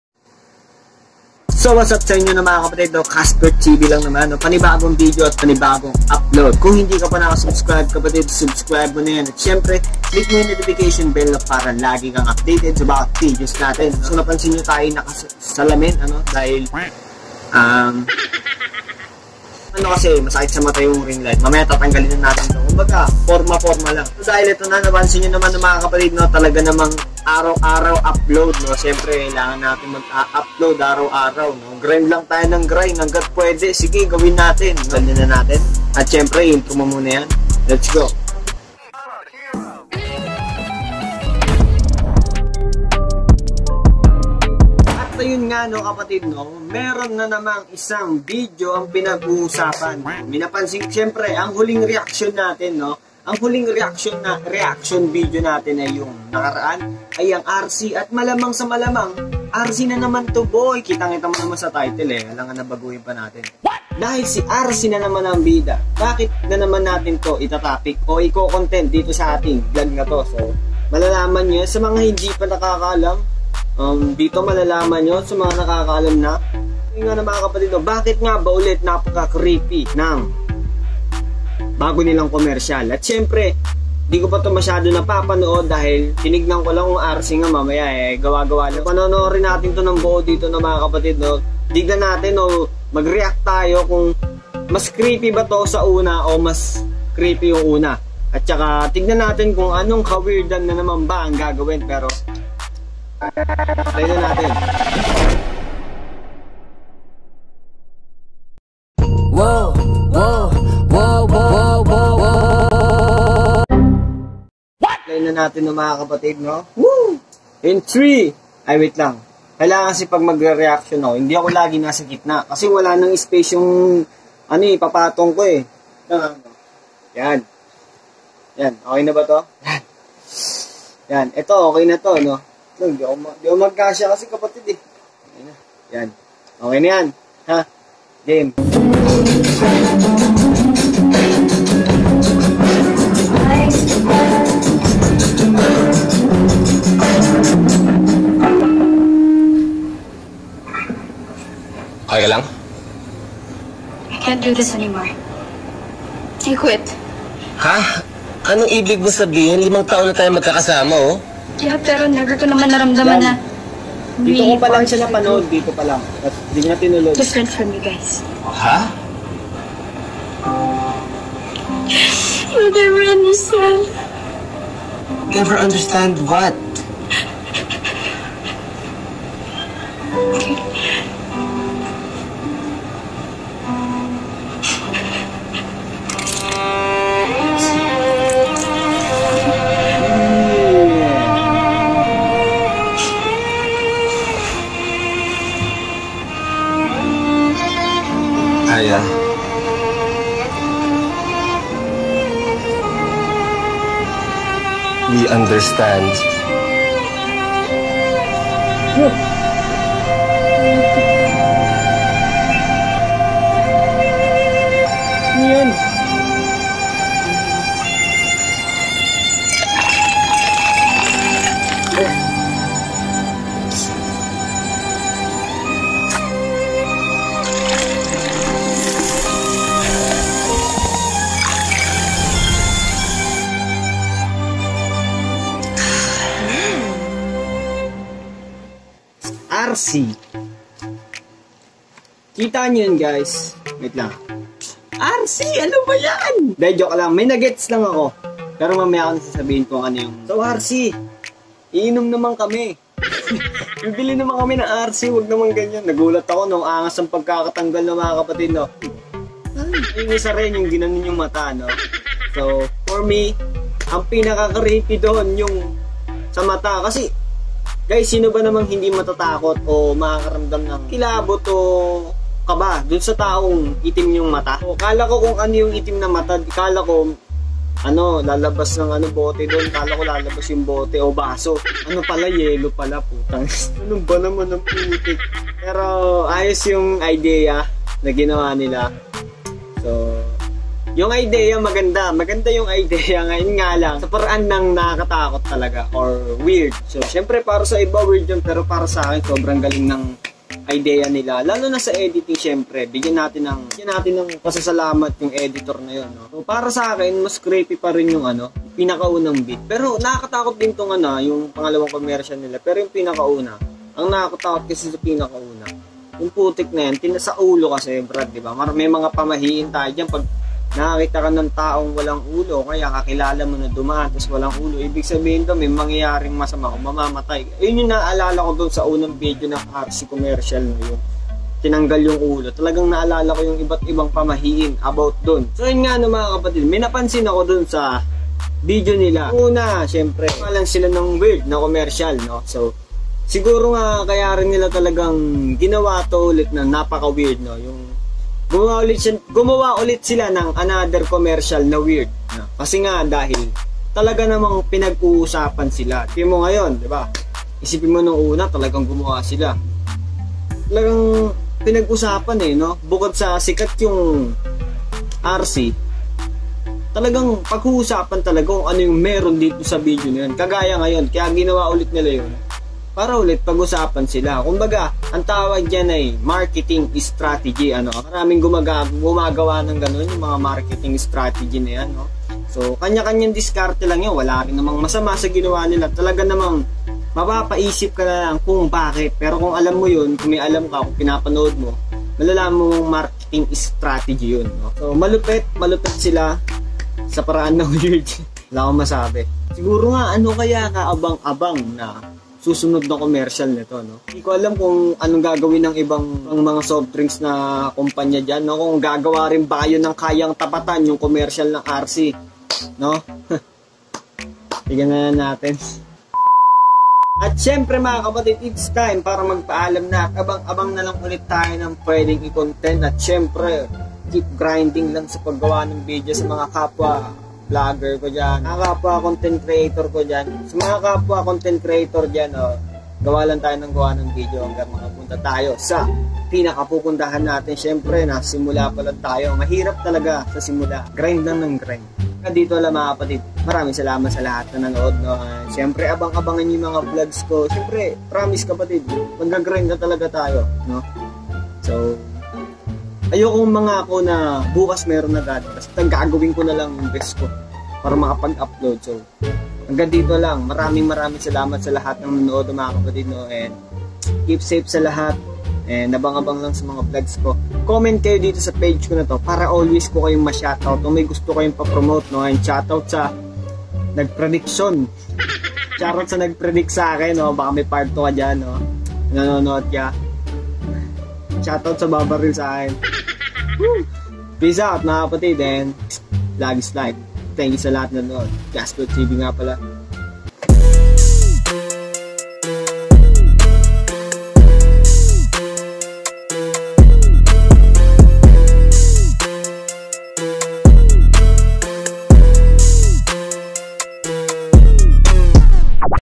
RC COLA COMMERCIAL | PART 2 | Mas creepy nga ba?
Another RC COMMERCIAL na reaksyon nanaman, dahil nga usap usapan nanaman amg commercial nila sa social media patungkol sa weird nitong kwento.